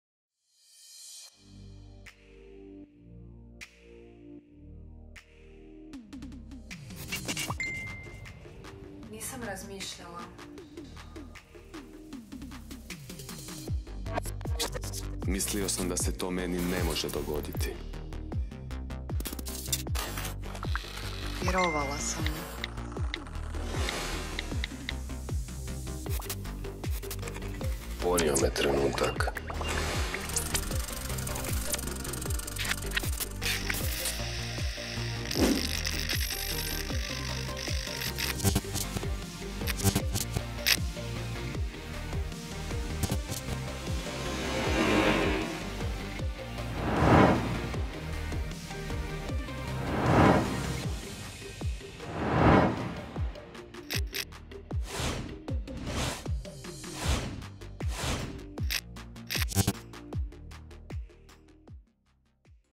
Meni se to ne moze dogoditi youtube
MeniSeToNeMozeDogoditi - najčešći je izgovor za rizično spolno ponašanje što može dovesti do neželjenih posljedica. Je li to i tvoj izgovor?
Kampanja „Meni se to ne može dogoditi“ povodom obilježavanja Svjetskog dana AIDS-a i važnosti spolnog zdravlja provodi se u informativne i obrazovne svrhe, s ciljem poboljšanja informiranosti i razumijevanja važnosti odgovornog spolnog ponašanja, zdravog stila života i zaštite od spolno prenosivih infekcija, a sve u cilju očuvanja i unapređenja zdravlja. Sadržaji na pregledan i zanimljiv način pružaju korisne informacije o spolnom i reproduktivnom zdravlju, dostupnim uslugama u zajednici i institucijama, uključujući prevenciju, testiranje i liječenje spolno prenosivih bolesti, sukladno važećim znanstvenim i stručnim spoznajama.